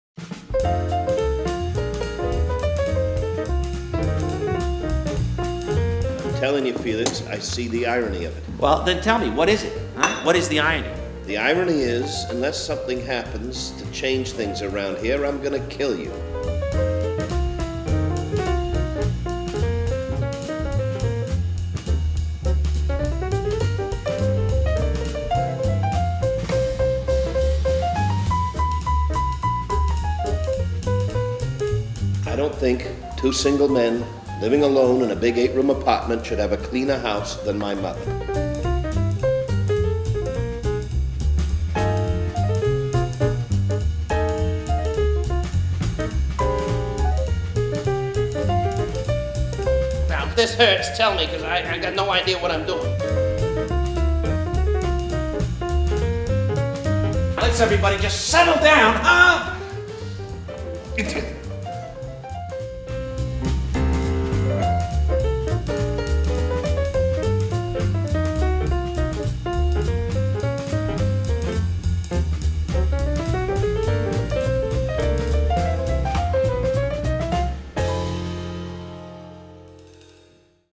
The Odd Couple (2016) - Trailer
Neil Simon’s legendary exploration of the relationship between fastidious Felix and his sloppy roommate Oscar is one of the most iconic and enduring pieces of 20th-century American comedy. Founding Member Diego Matamoros and Artistic Director Albert Schultz reprise the title roles as the suddenly single pals who become roommates with hilariously disastrous results.